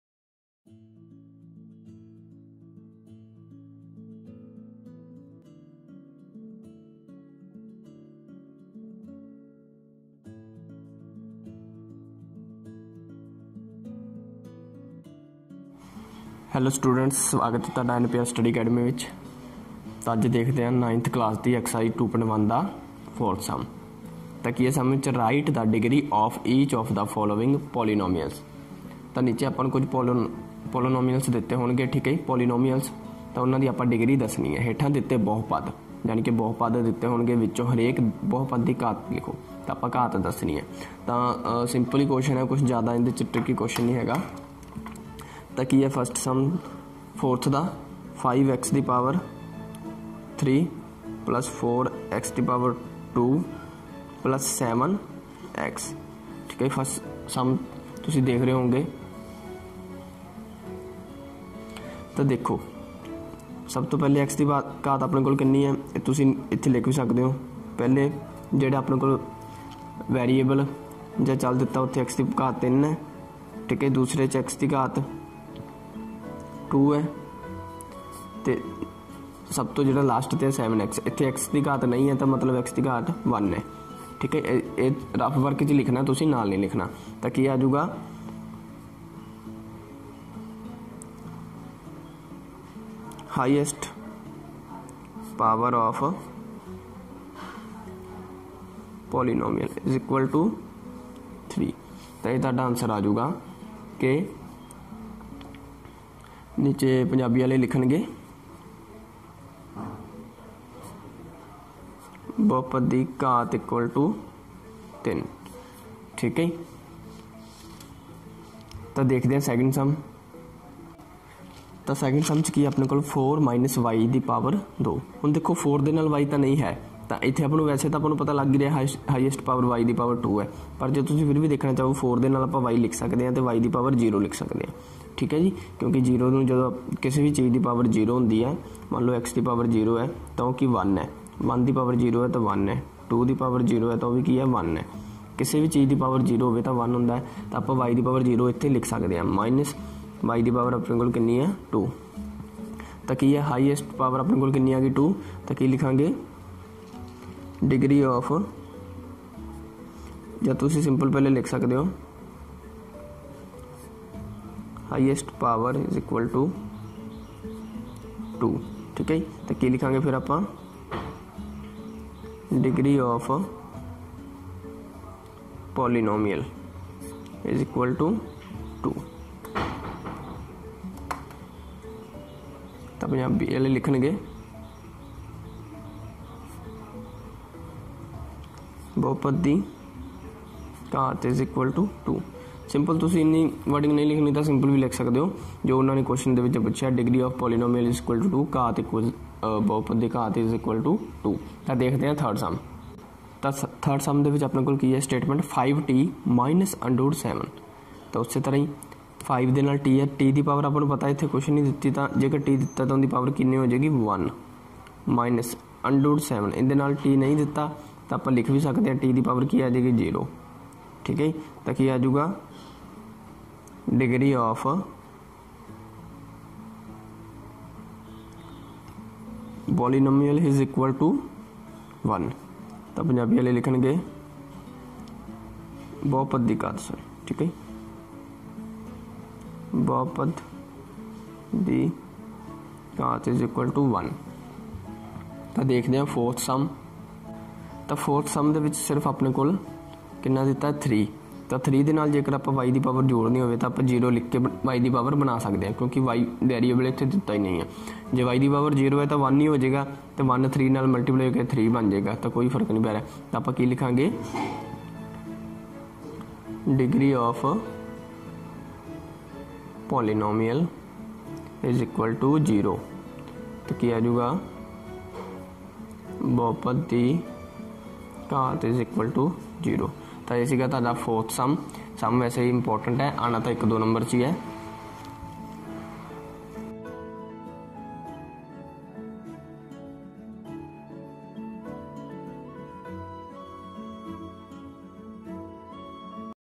Class 9th Exercise - 2.1 Sum no:- 4 (Degree of polynomial) in punjabi and hindi PSEB/CBSE | NCERT
Class 9th Exercise - 2.1 Sum no:- 4 (Write the degree of each of the polynomial) in punjabi and hindi PSEB/CBSE | NCERT

Class :- 9th